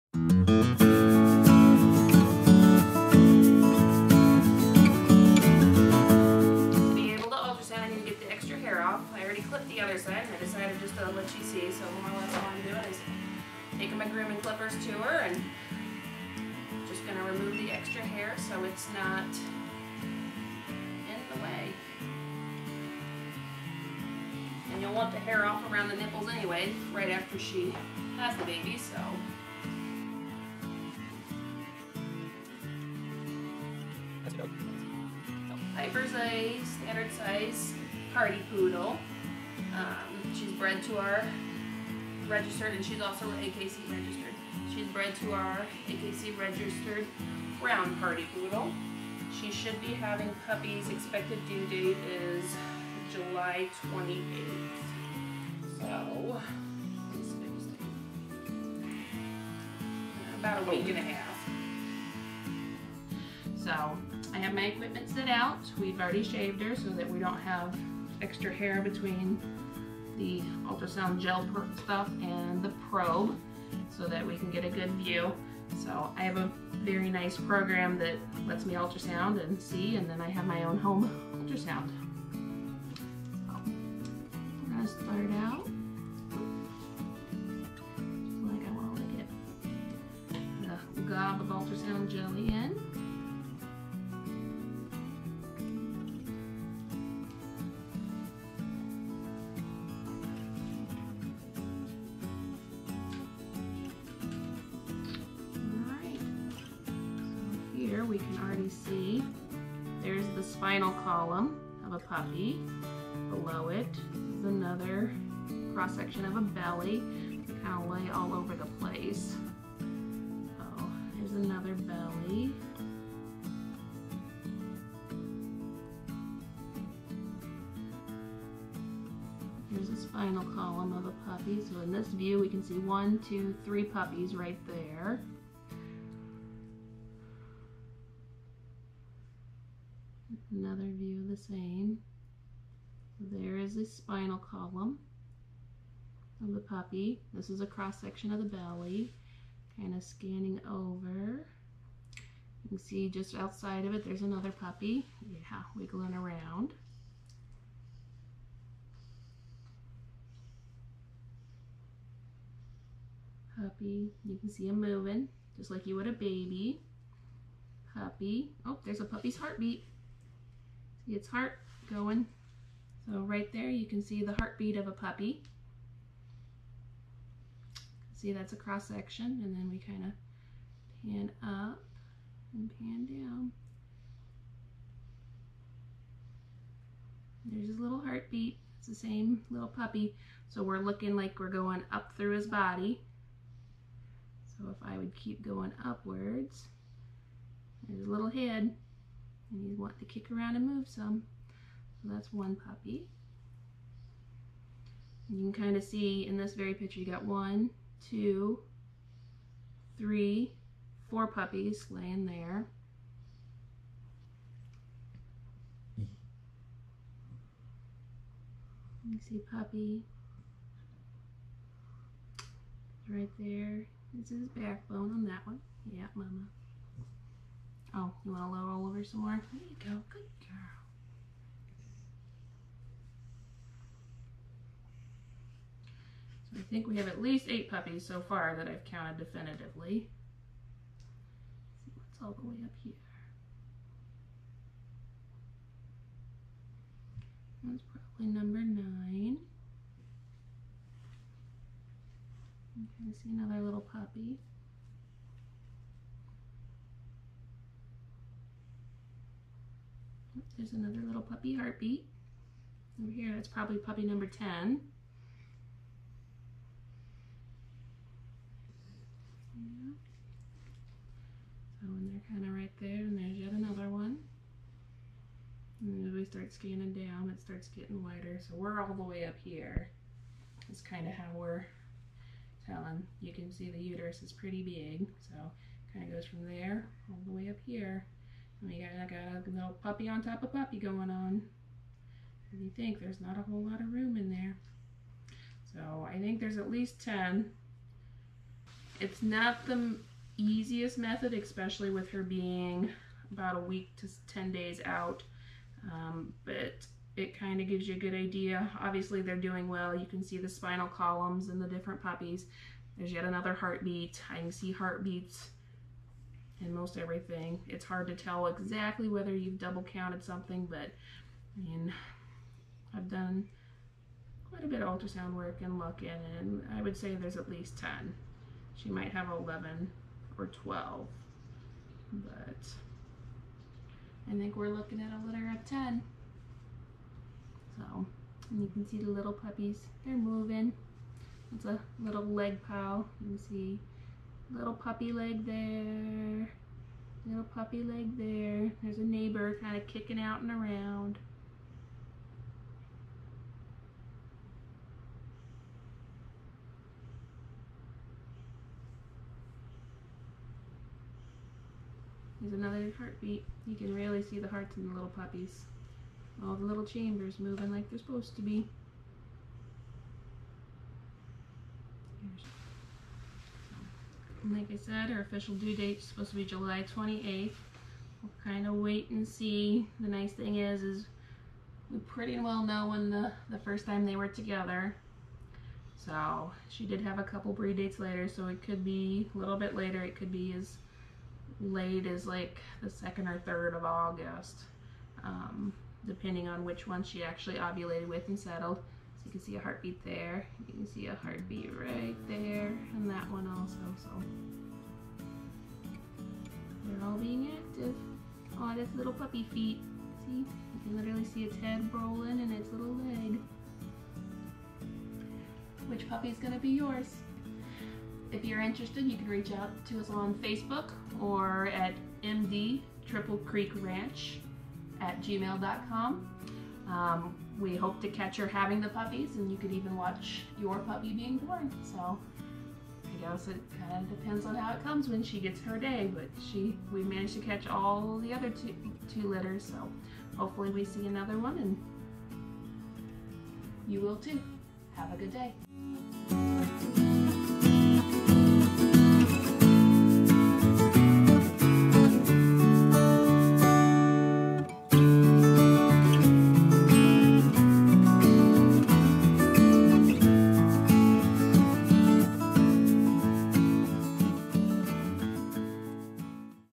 Poodle Ultrasound
Piper is our female standard poodle and in this video we are using an ultrasound that plugs into an iPad to ultrasound her about a week before she is due to have her summer of 2020 litter. I had planned not to have this litter just because the twins being 19 months old are a handful enough, but they had other plans. Since the girls have learned to open the poodles crates and they let Rollo and Piper out together, we decided we would have a litter this summer/fall. Every thing so far is going well and it will be fun to watch the babies play with the puppies when they get here.

If you are interested in a puppy from this litter or a future litter of standard poodles, you can reach out to us here, or find us on Facebook (MD Triple Creek Ranch) or our email